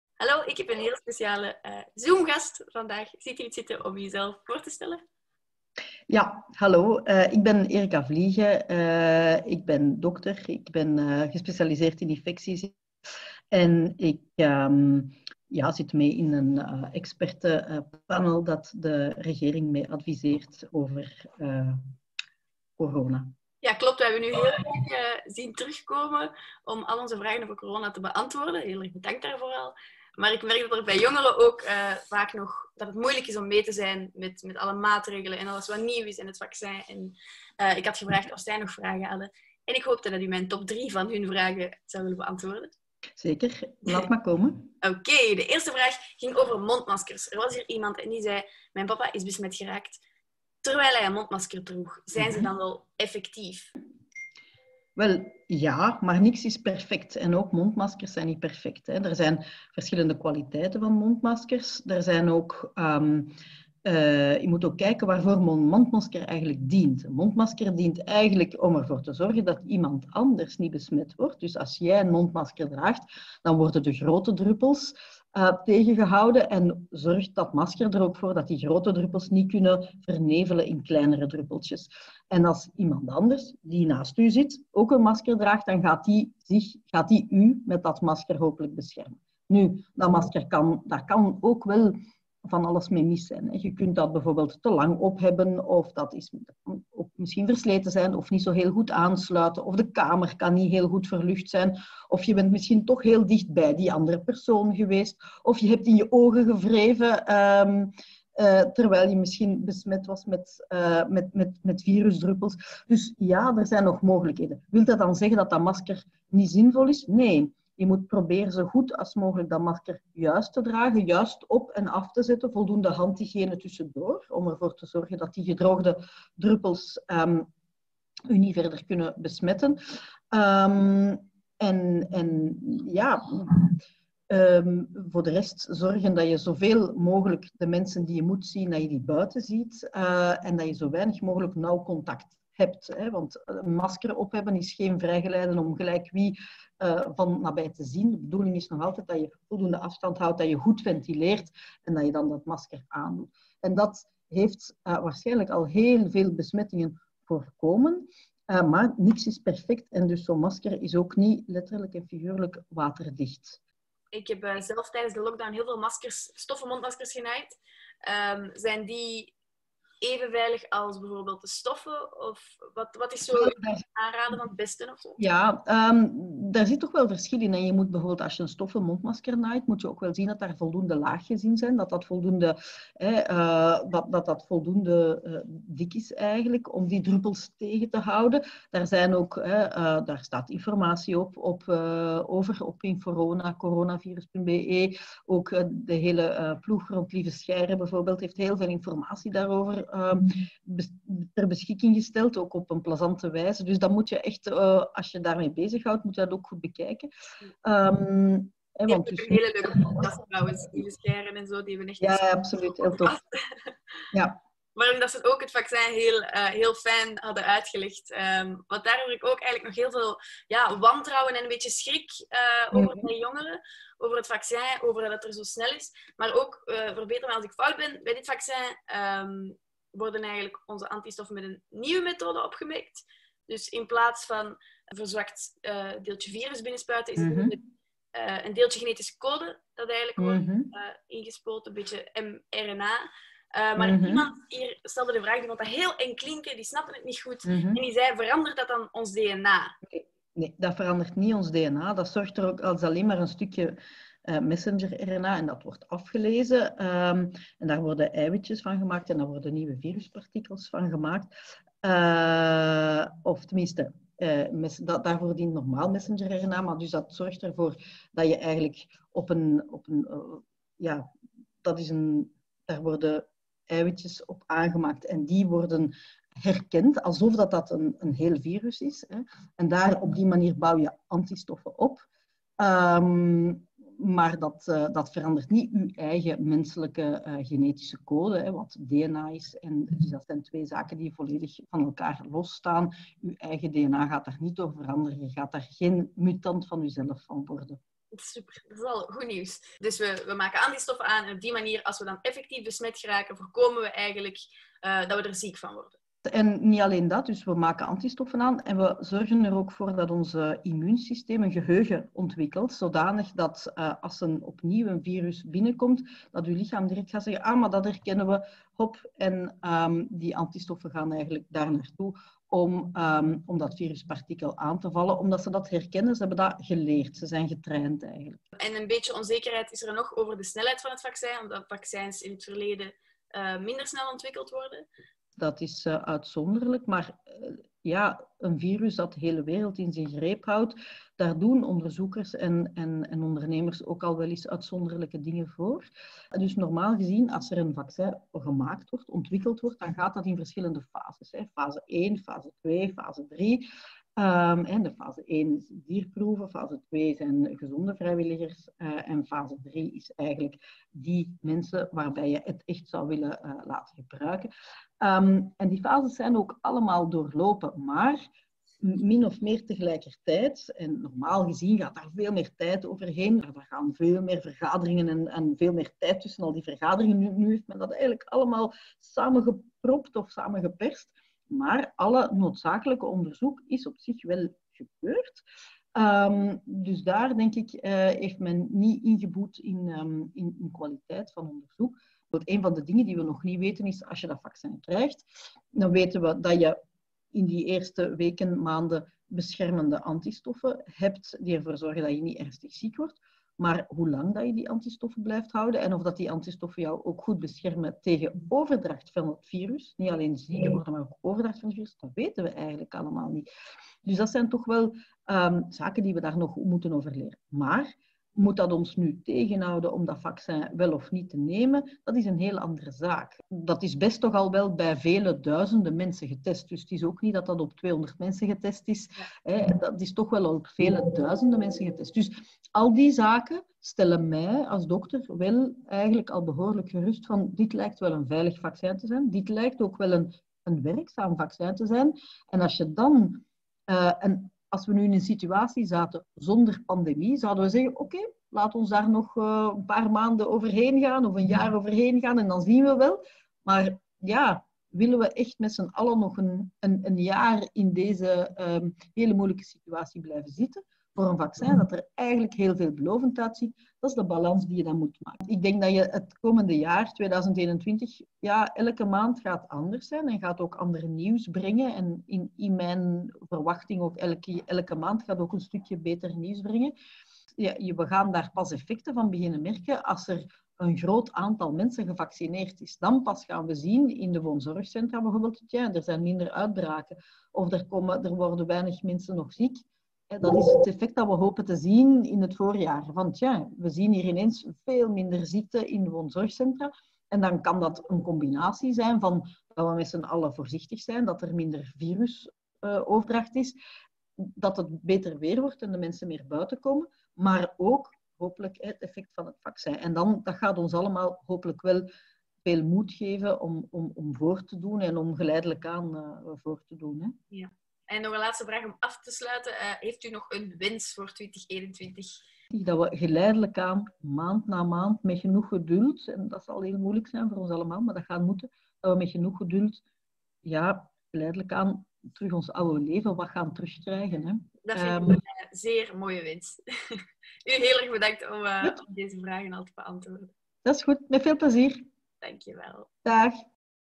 Pommelien Thijs interviewt Erika Vlieghe over coronamaatregelen
Waarom raakte mijn papa besmet, terwijl hij wel een mondmasker droeg? Met deze vraag start Pommelien haar gesprek met Erika Vlieghe. Wat ze nog meer vroeg? 👀 de video.

Meer weten over de gemaskerdescholieren? Check